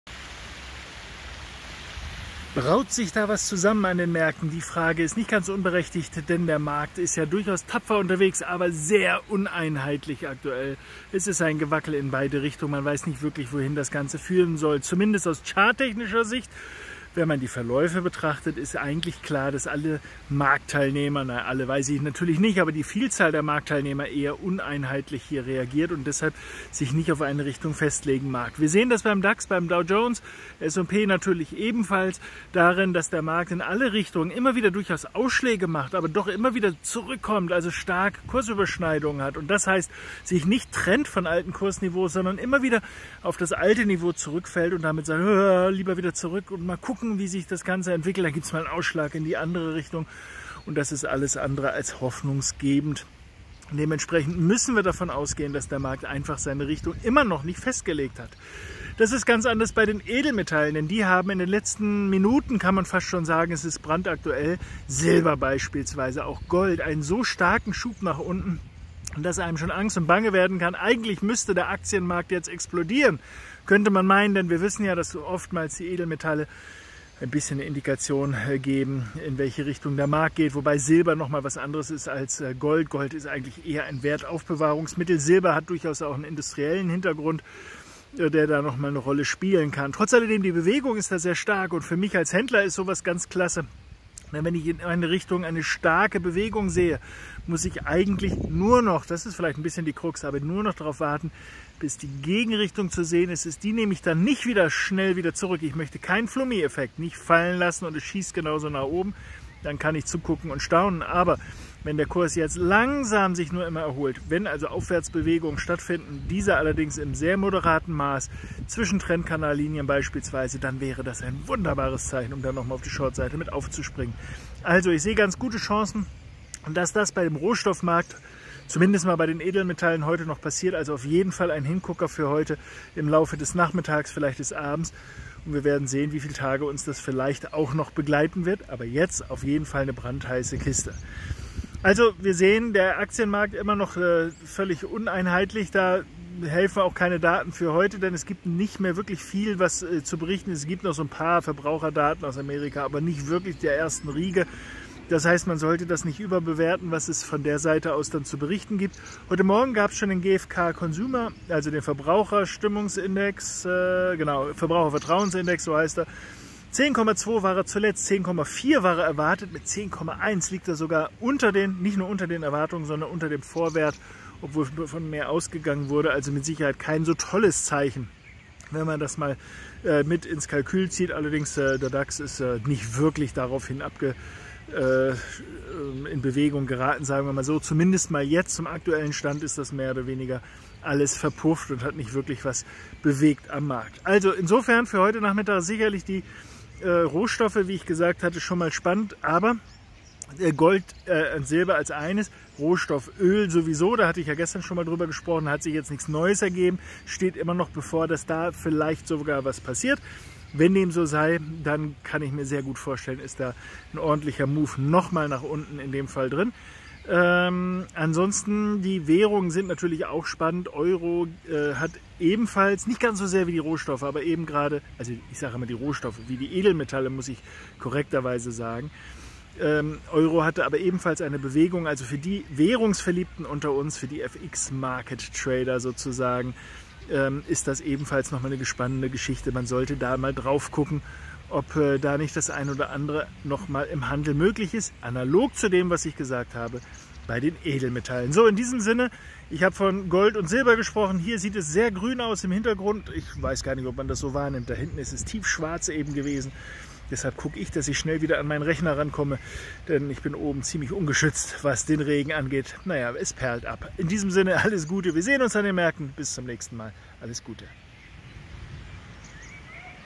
Rüdiger Born: Gold und Silber mit starkem Abwärtsschub
Dax und US-Indizes bringen zwar Ausschläge, kommen dann aber wieder zurück. Hoffnungsgebend ist das nicht für einen klaren Trend. Silber und Gold bieten aktuell einen starken Schub nach unten. Kann man hier noch Short aufspringen? Rohöl und Devisen spreche ich auch an. Mehr dazu im Video. Nein? Dann einfach hier anmelden, es ist kostenlos!